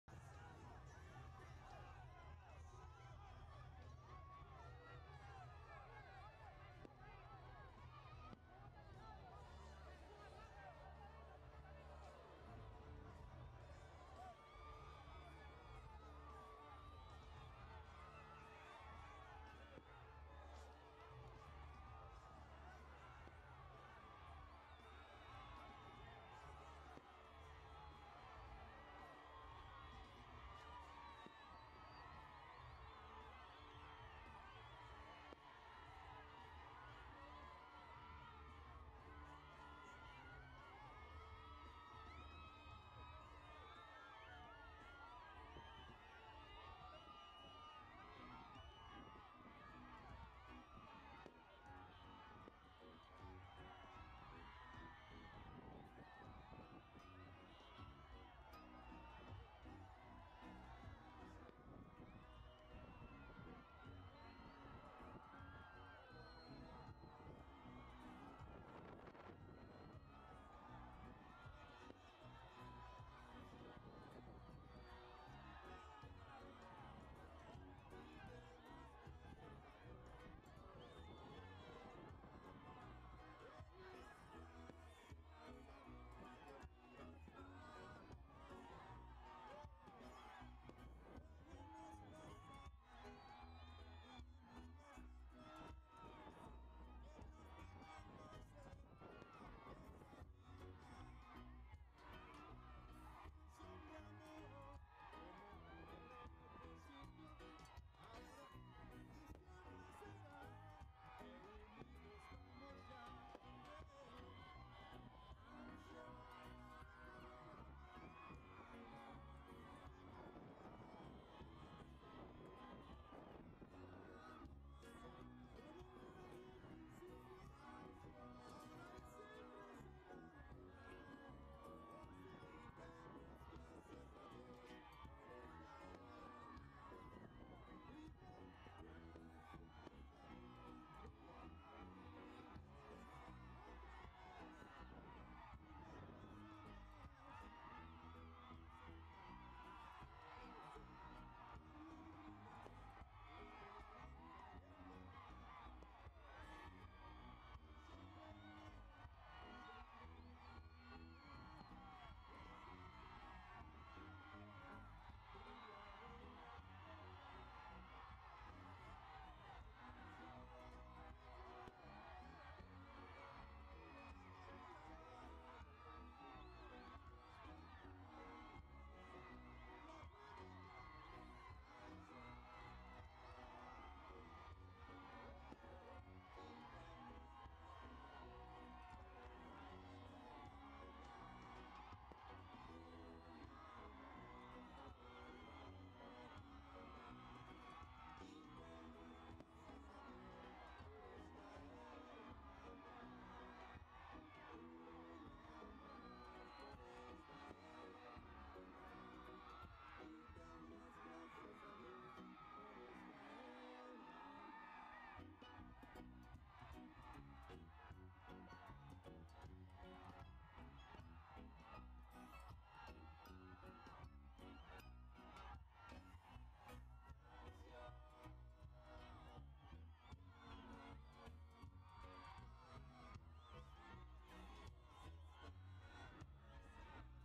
Entrada del Comandante Daniel Ortega y Compañera Rosario Murillo a la plaza de la fe en 2018
Así fue la entrada del Comandante Daniel Ortega y la Compañera Rosario Murillo a la plaza La Fe, para conmemorar el 39 aniversario de la Revolución Popular Sandinista en Managua.

La marcha hacia las victorias no se detiene… Siempre Sera 19!